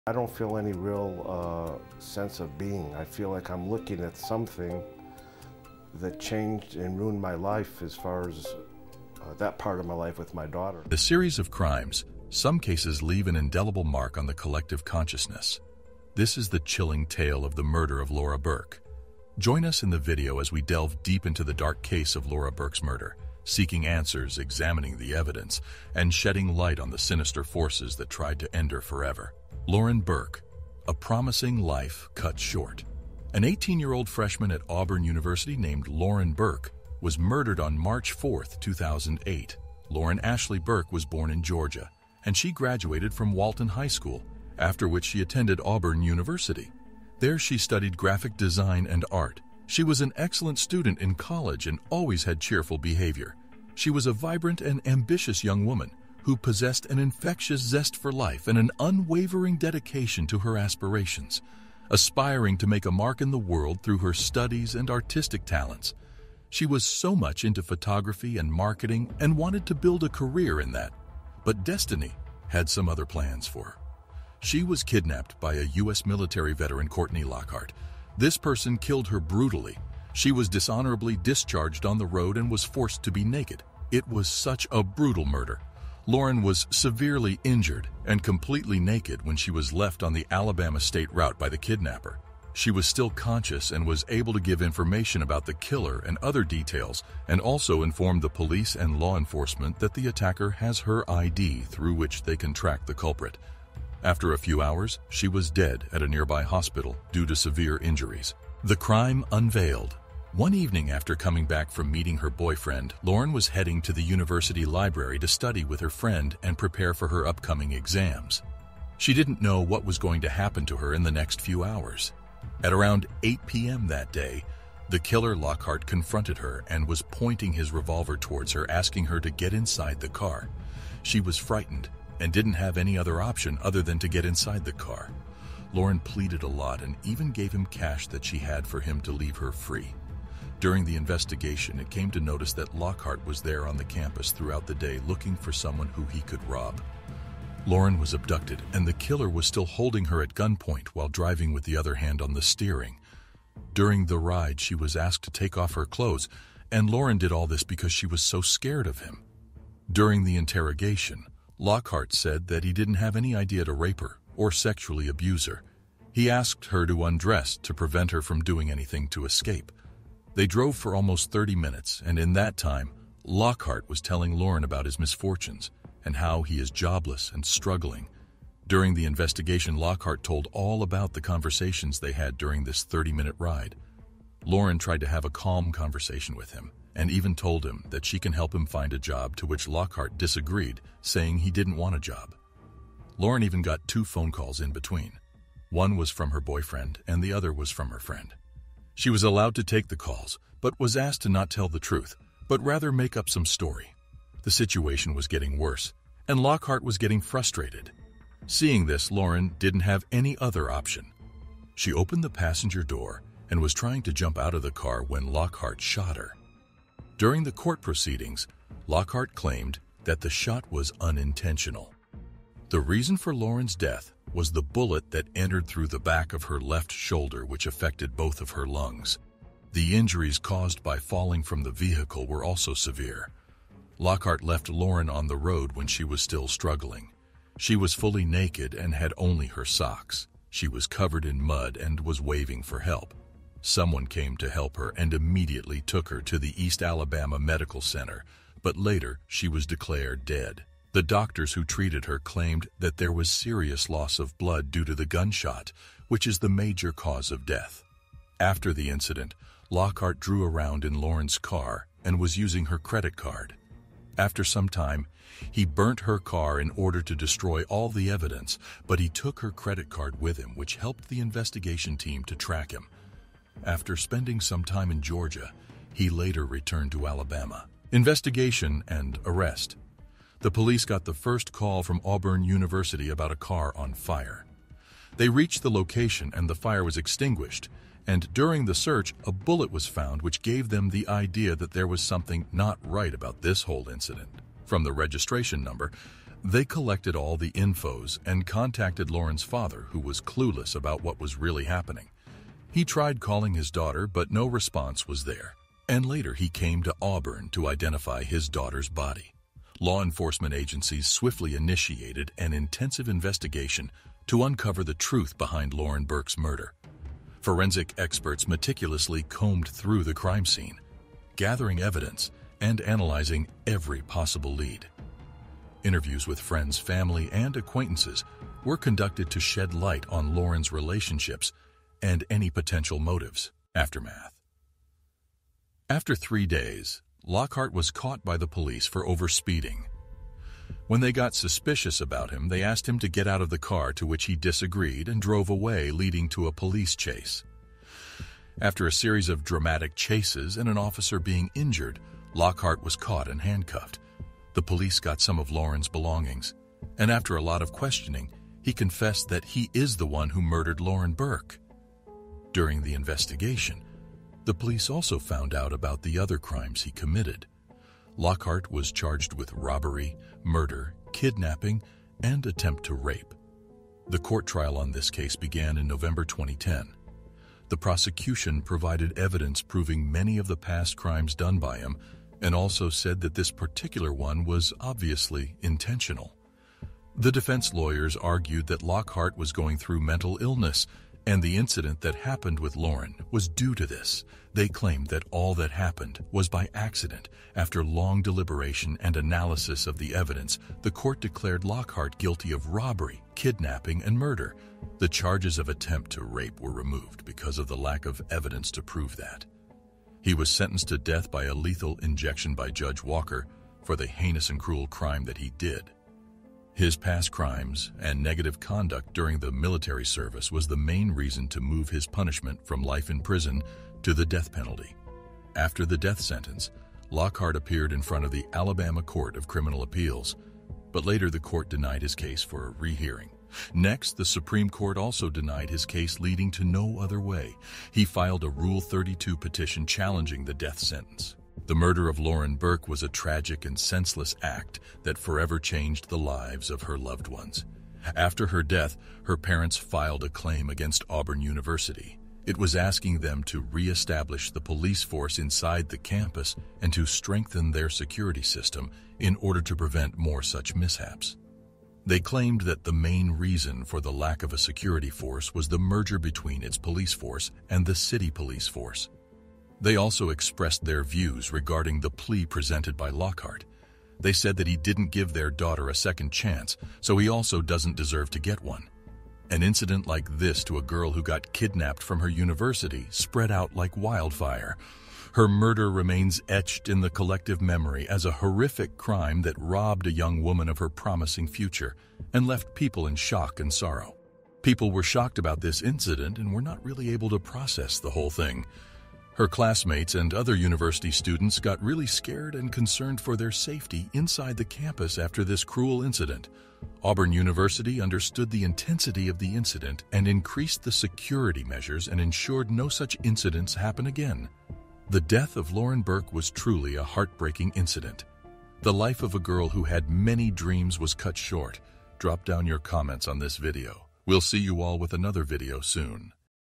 The DEVASTATING Story of How Lauren Burk Was BRUTTALY Burned
The Devastating Account of How Lauren Burk Was Brutally Burned

In the series of crimes, some cases leave an indelible mark on the collective consciousness. This is the chilling tale of the murder of Laura Burk. Join us in the video as we delve deep into the dark case of Laura Burk's murder, seeking answers, examining the evidence, and shedding light on the sinister forces that tried to end her forever."